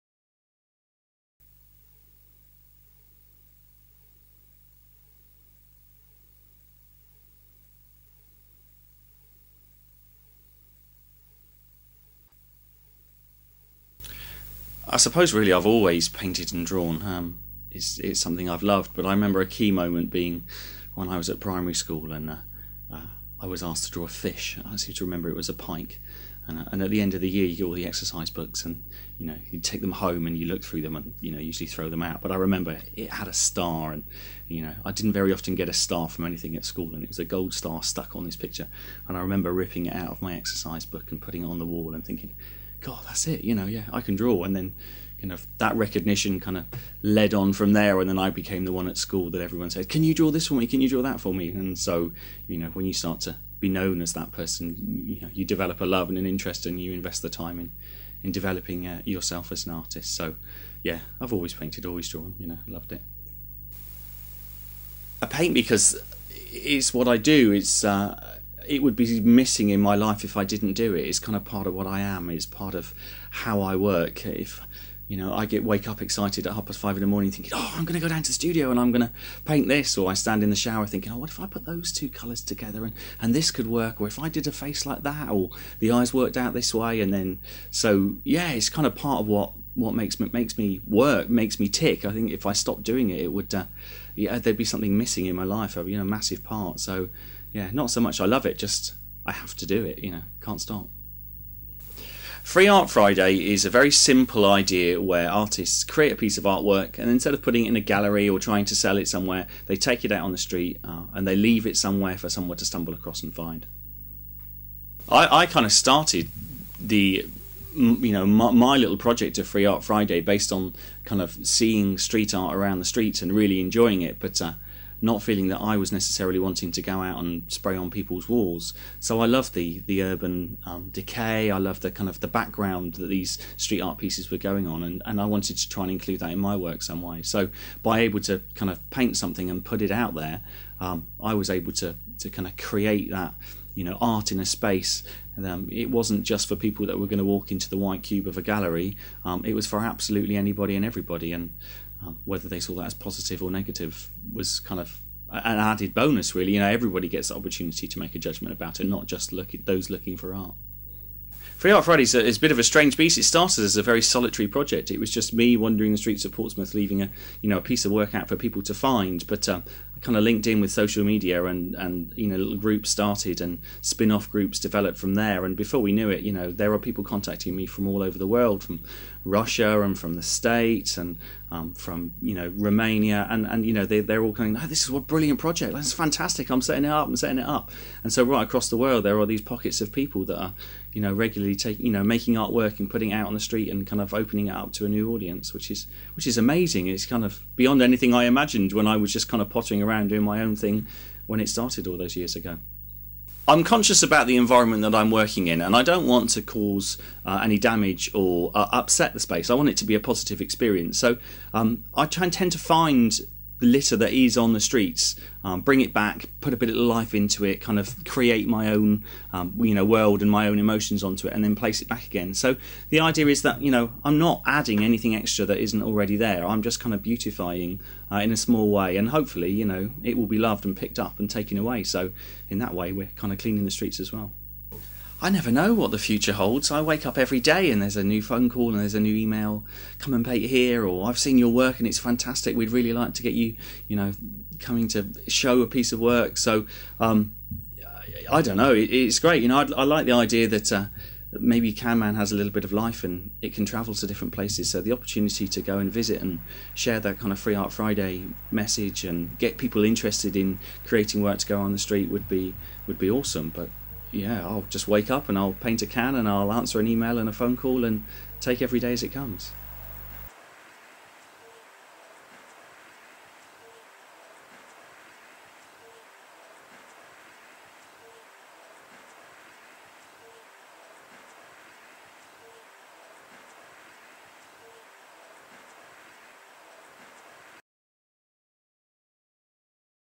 My Dog Sighs - Walk by, Ignore me, Forget you ever saw me
10 years ago, UK artist My Dog Sighs started leaving his artwork on the streets of Portsmouth. Anyone passing by could take them - for free.

Now, some of his artwork will be sold for hundreds of pounds at his solo exhibition show at the Westbank Gallery in London from March 21-31, 2013.

41-year-old My Dog Sighs, who recently quit his teaching job to focus solely on his passion, tells me his story.